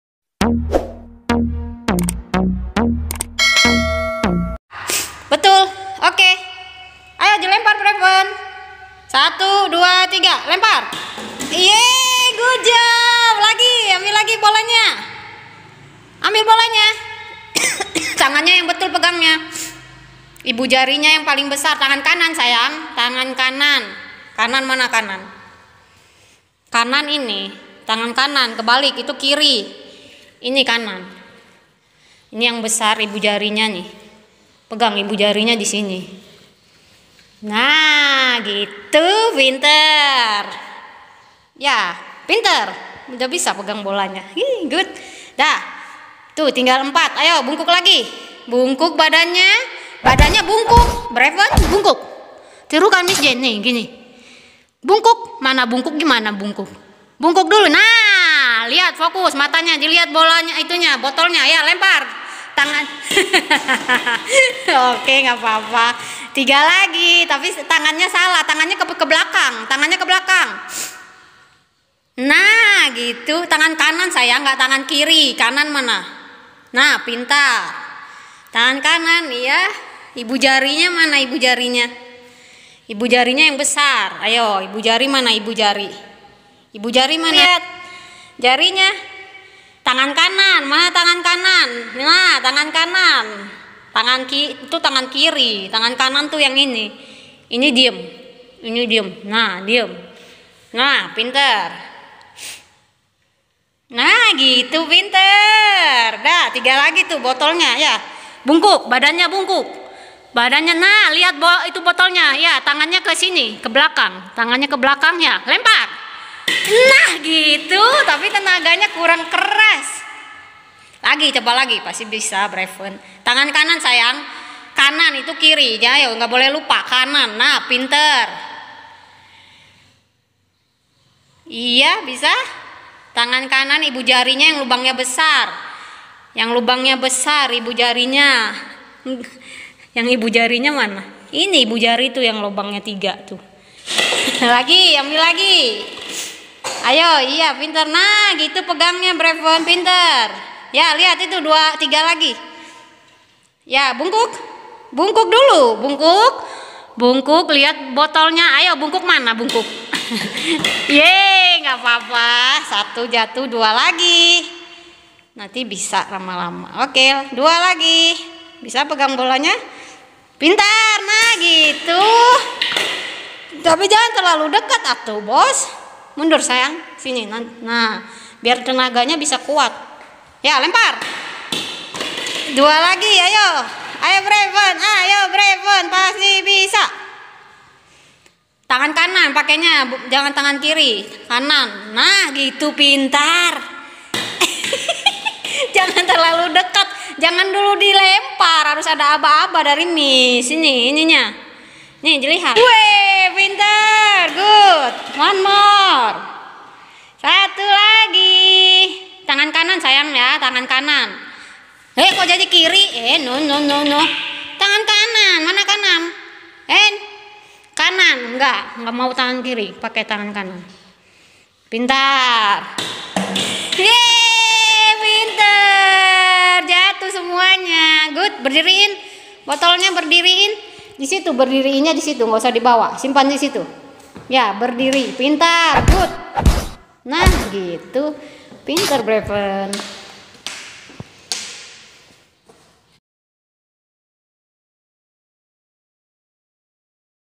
📌 WAJIB MILITER ANAK HappyHolyKids, UNTUK LATIHAN MOTORIK KASAR & KONSENTRASI 😁🤗@JennsDaily ⁉️
📌 WAJIB MILITER ANAK AJAIB HappyHolyKids, UNTUK MELATIH MOTORIK KASAR & KONSENTRASI  😁🤗 ⁉️ Preschool !! School vlog ⁉️ Kls.Inklusi ⁉️ Play & Learn !! HappyHolyKids !! Jenn'sDaily

Hello blessed mom and dad 🤗☺
Semoga sehat2  ya... 🙏🙏🙏